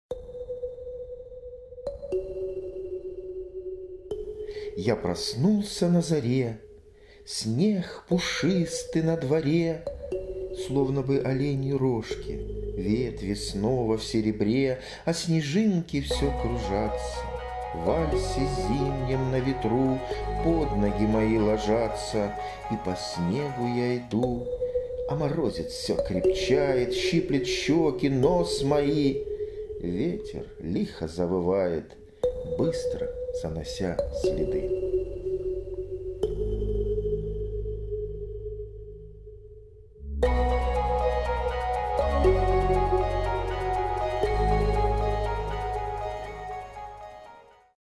"Я проснулся на заре" стихотворение В. Гаазова
Василий Гаазов читает своё стихотворение "Я проснулся на заре".